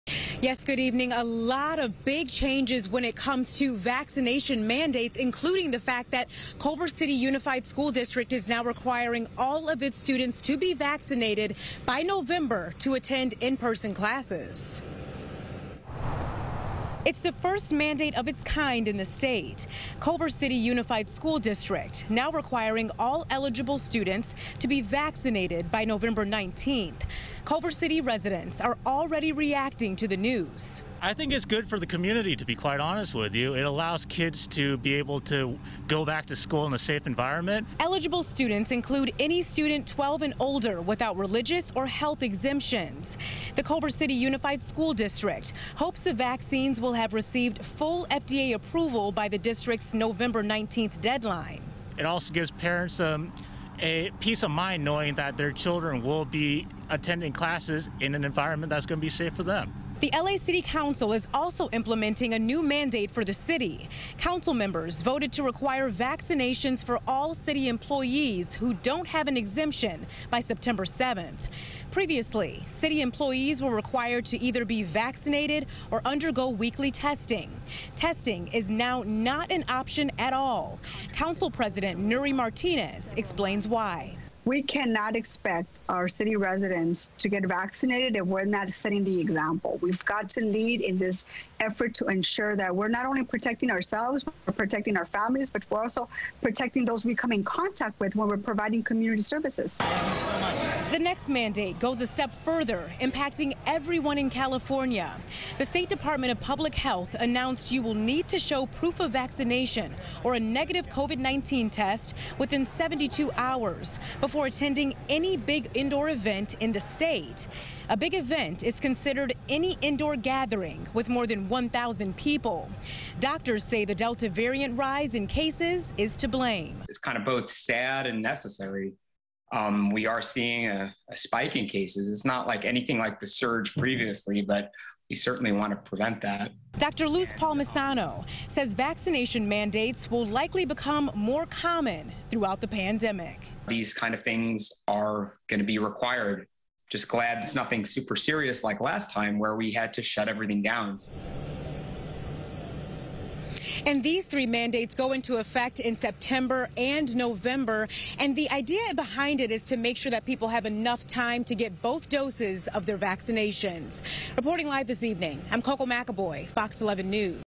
Culver City Unified to require eligible students, teachers to get vaccinated
Eligible students and teachers at Culver City Unified School District will soon need to provide proof of vaccination.